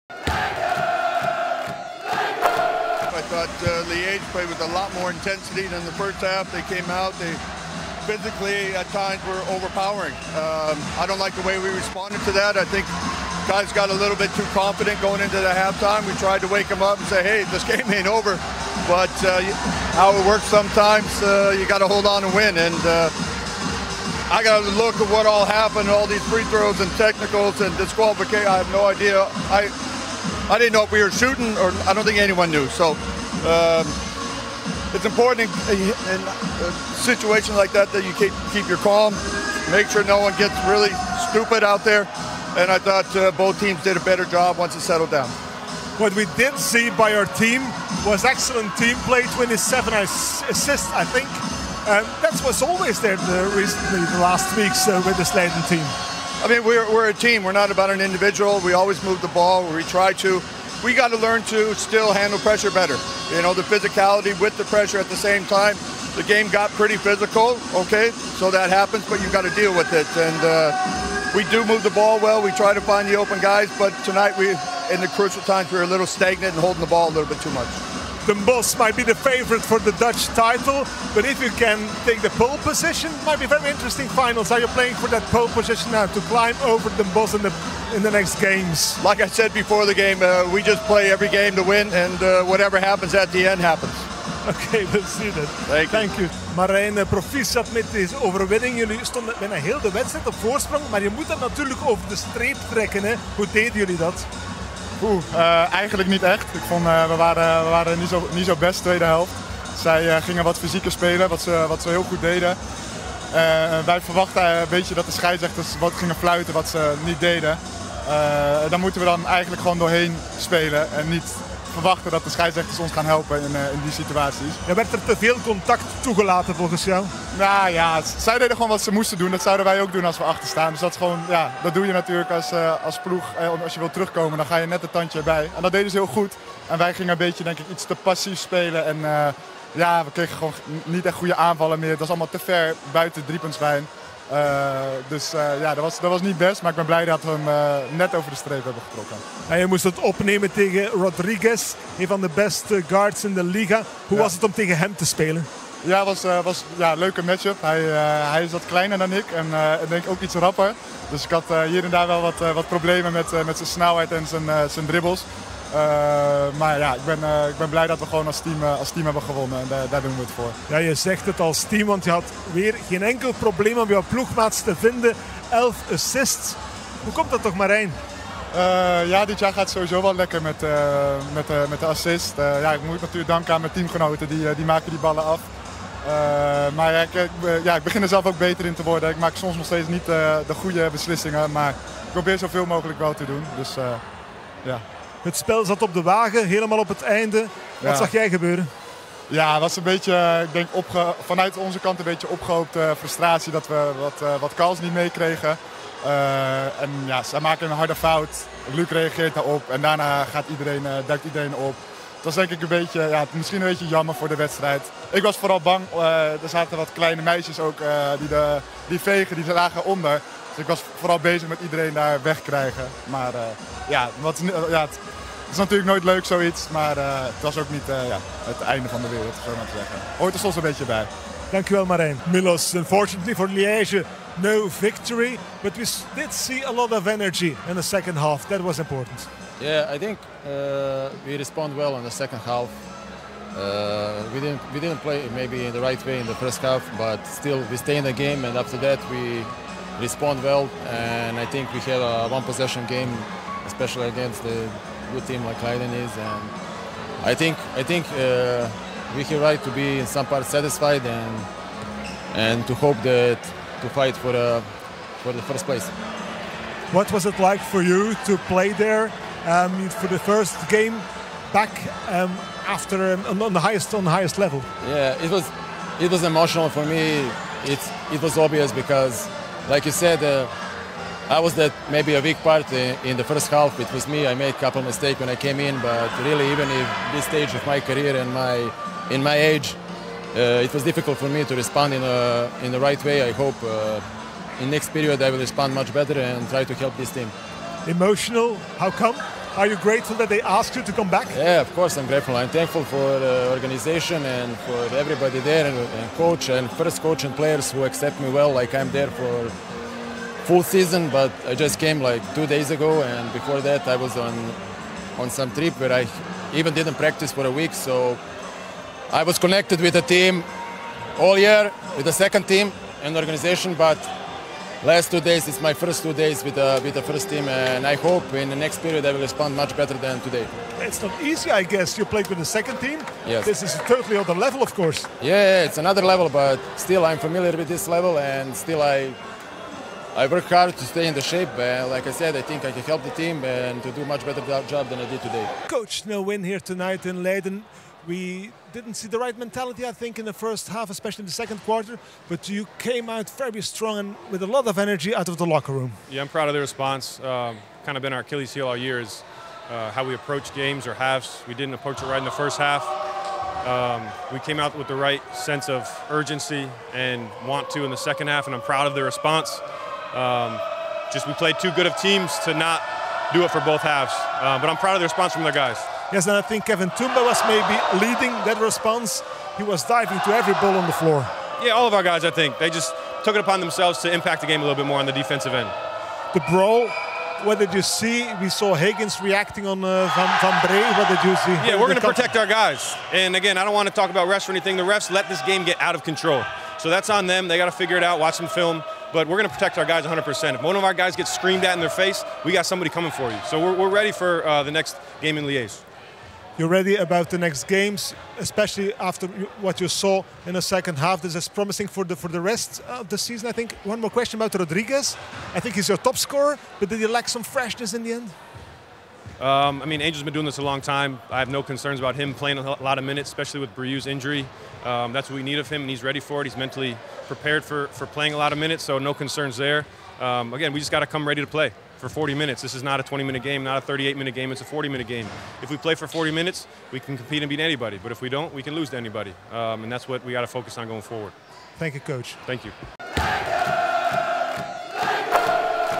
Interviews na Zorg en Zekerheid Leiden - RSW Liège Basket, BNXT League (30 mrt 2024)
Na afloop van de Leidse winst op Liège (Luik) spreekt Maarten Weynants met beide hoofdcoaches, Doug Spradley (Leiden) en Alex Zampier (Luik), met Zorg en Zekerheid Leiden captain Marijn Ververs (8p, 7r, 11a, 1s, 1b) en met guard Milos Bojovic van RSW Liège Basket.